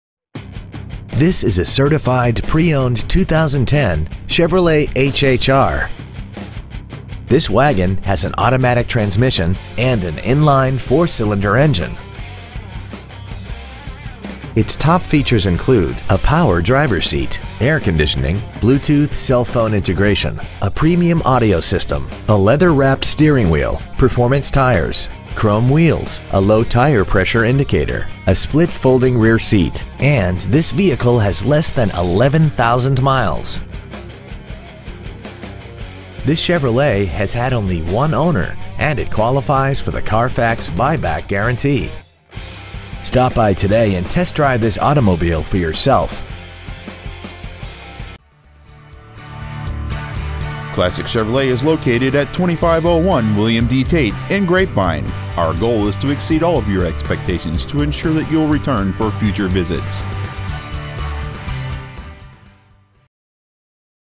2010 Chevrolet HHR Dallas TX
Year: 2010
 Make: Chevrolet
 Model: HHR
 Engine: Gas I4 2.4L/146
 Trans.: Automatic
 Exterior: Arctic Ice
 Miles: 10,736
 Interior: Black

 2010 Chevrolet HHR Grapevine TX